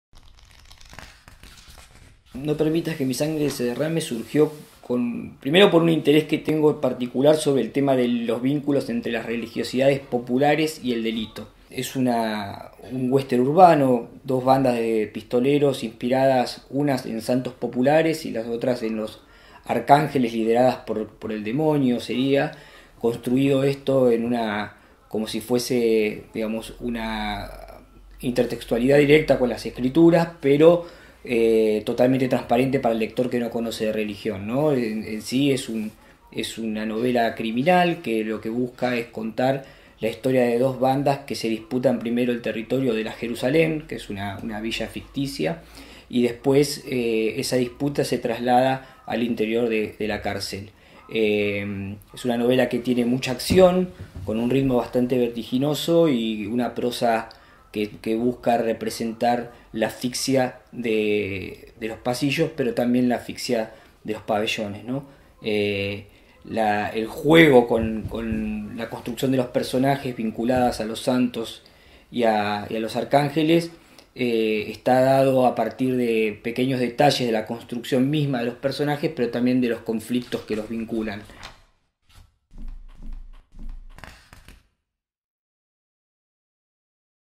Juan Carrá anticipa su novela No permitas que mi sangre se derrame
El duelo a muerte signado desde la infancia de dos criminales: uno que vive como delincuente, el otro como policía.